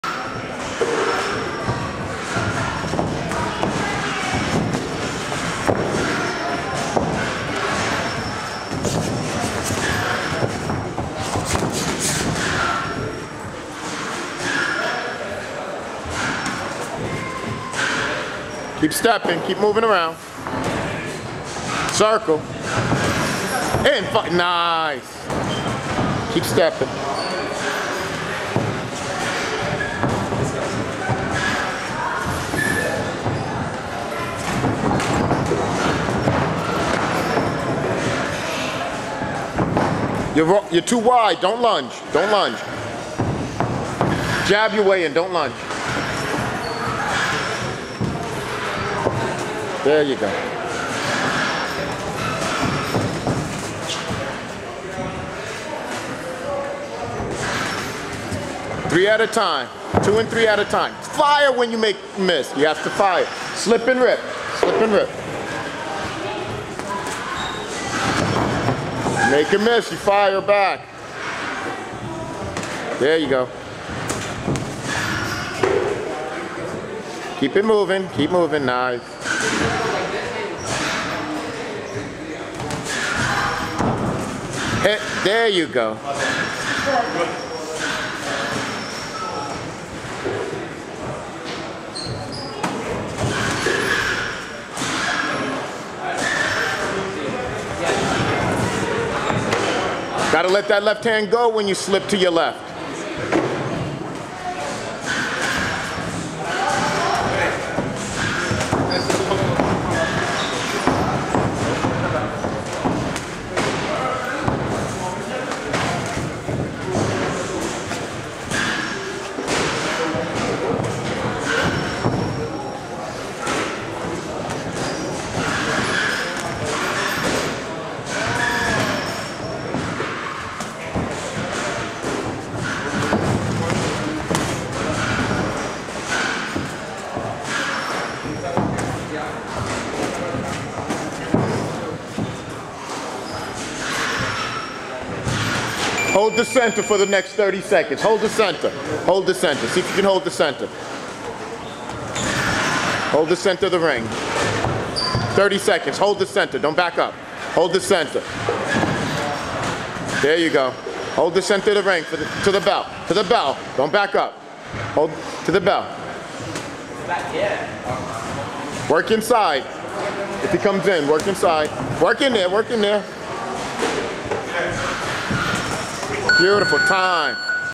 BROOKLYN FIGHT CLUB GLEASON'S GYM 11-20-11 ROUND 2
Box spar at Gleason's
Brandon Johnson vs David Sjursen
Trainer - Malin Ali Bey